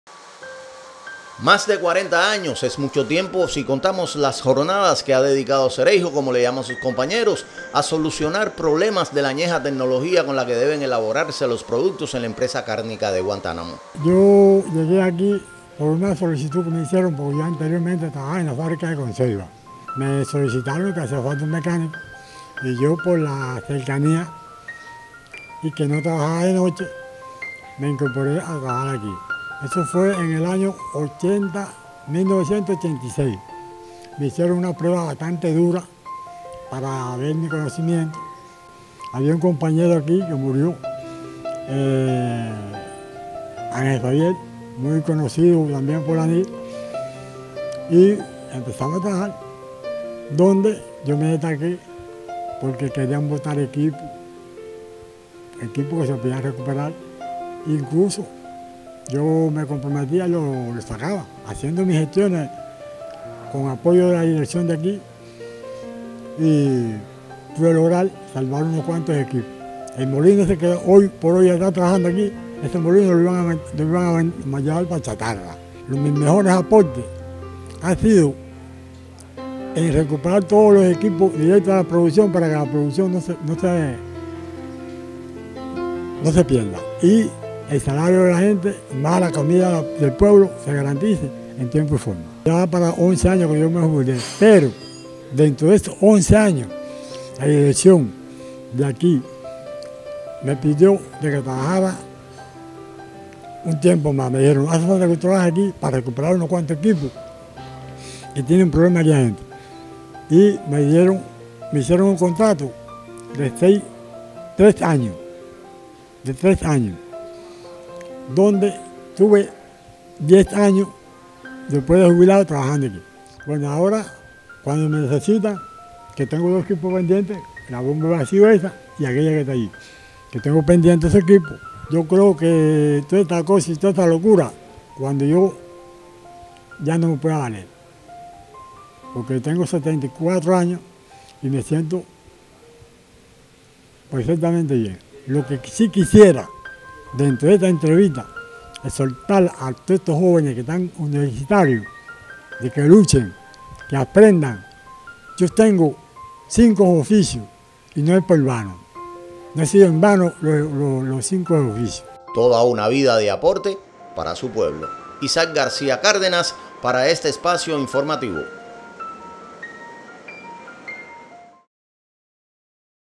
Cereijo: Más de la mitad de la vida creando…
Para Ricardo Cereijo Abad resolver problemas tecnológicos en la Empresa Cárnica de Guantánamo se ha convertido en su razón de ser… Este septuagenario guantanamero ha dedicado más de CUARENTA años a la labor anirista en ese importante centro productivo…